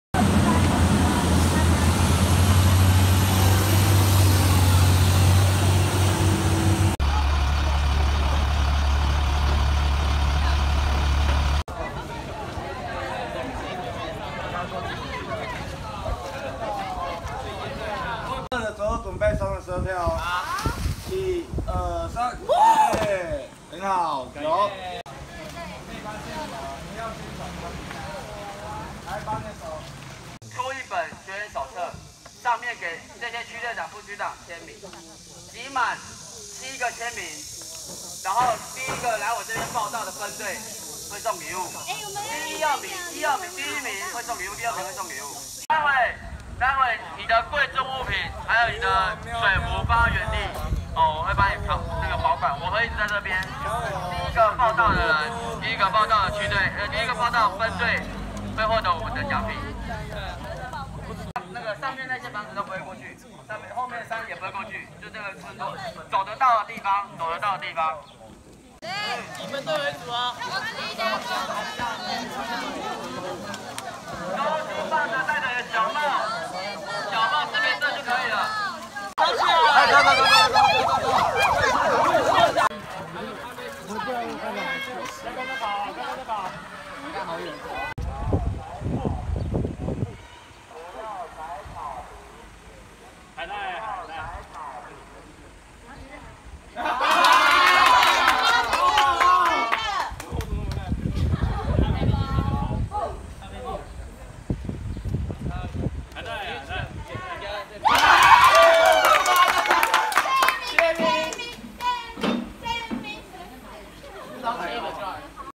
馬祖日報2018/07/18影音／「武」動青春 馬祖戰鬥營第2梯次18日熱情登場
日期：2018/07/18下午14:30
地點：津沙村
攝影：吳嘉榮
　馬祖戰鬥營第2梯次活動熱情登場，馬防部精心準備一系列課程，包含基本教練、兩棲戰技、威武巨砲、勇猛戰甲、神兵利器、百步穿楊等6項軍事課程，及醫護專業、戰地巡禮、團康活動等一般課程，除了親身體驗國防事務，還能暢遊在地美景，使學子豐富暑假生活。